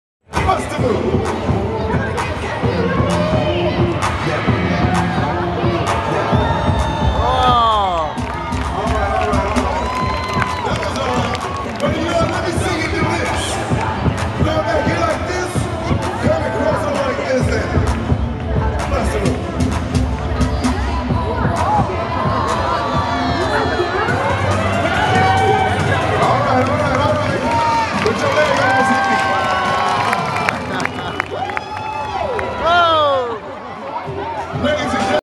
Stilt walkers performance
Stilt walkers during the circus  preshow